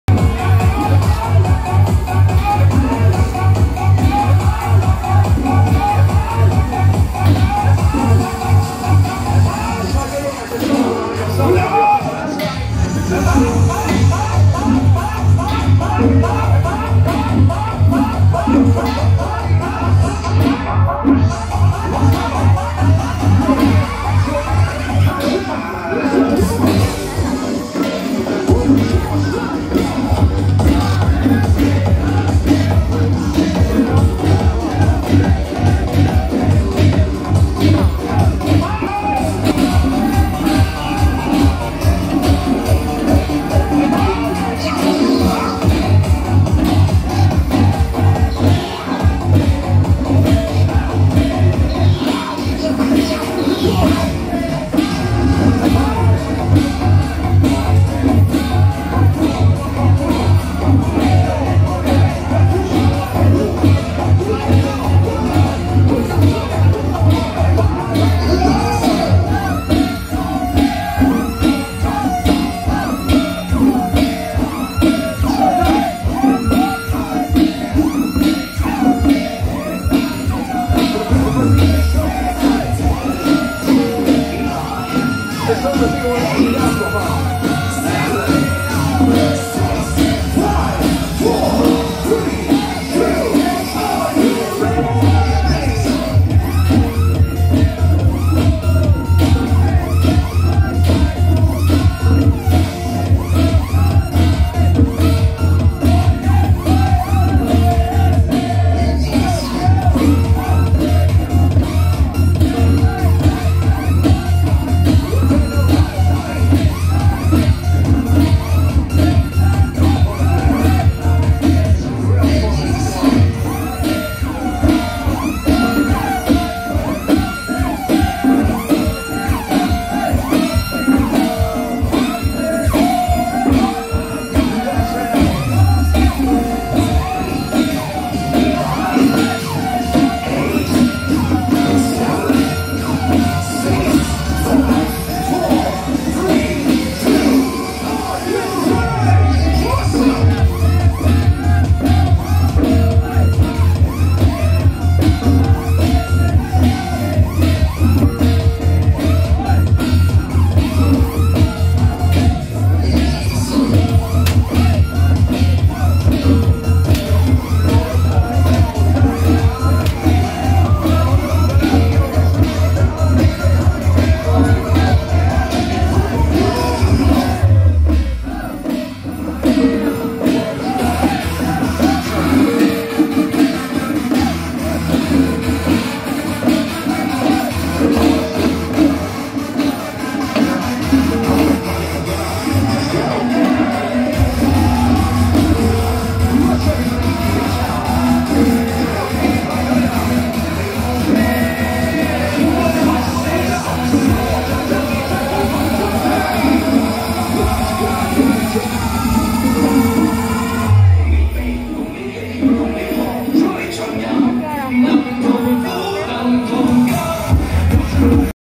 【台北協靈堂 閭山小法團】~111板橋無極弘道宮建宮12週年祛瘟除災祈福遶境
台北協靈堂 閭山小法團